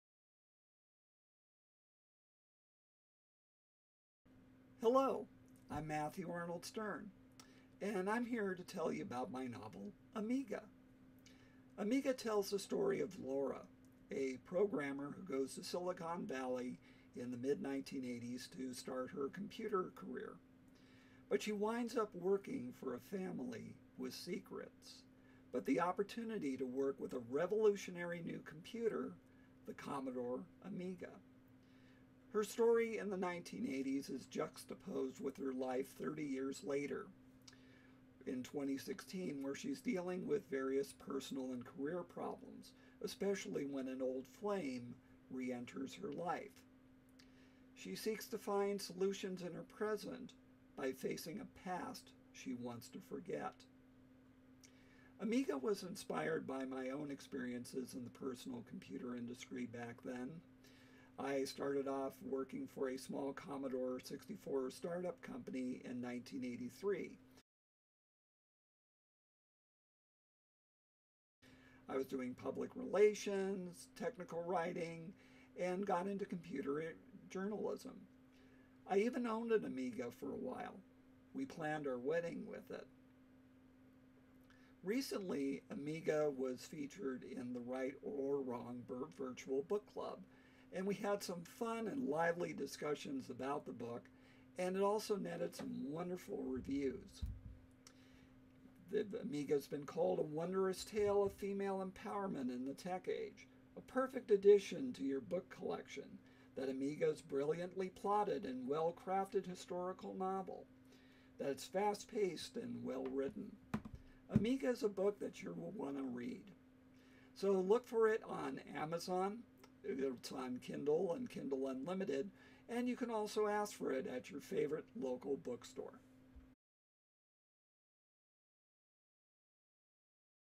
The Story Behind Amiga
Here's more about my novel Amiga, what inspired me to write it, and my experiences with the Write Or Wrong Virtual Book Club where Amiga was Book of the Month in August 2020.